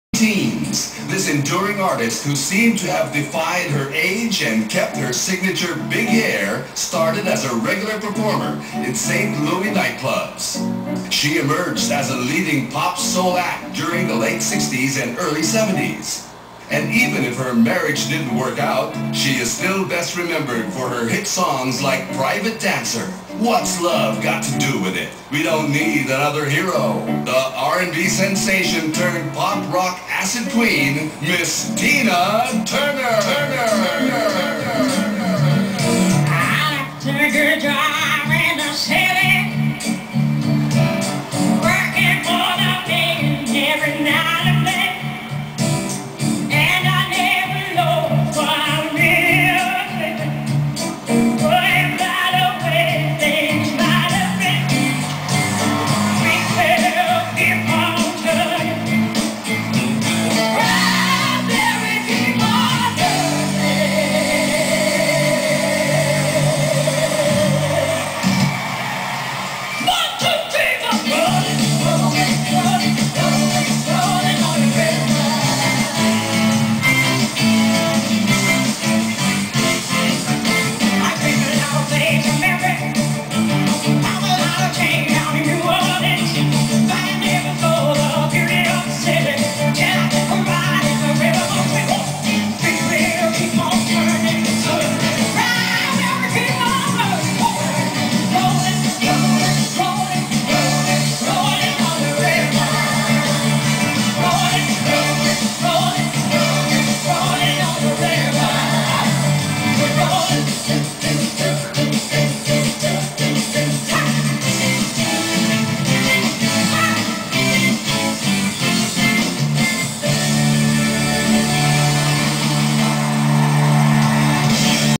Lani Misalucha as Tina Turner
Lani Misalucha on her 3rd major concert, Missing You.